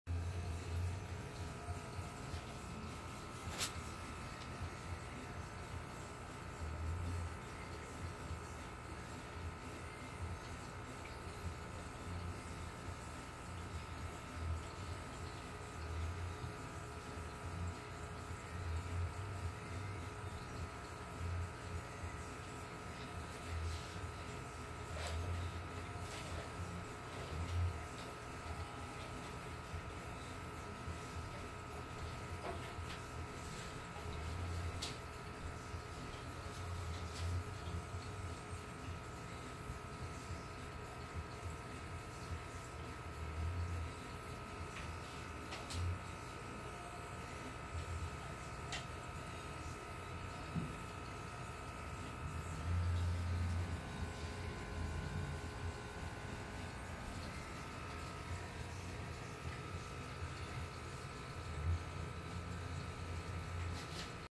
75 gallon planted Discus community tank update
My 75 gallon planted Disucs tank. The fish are now 5+ years old with addition of 5 new fish from a friends aquarium.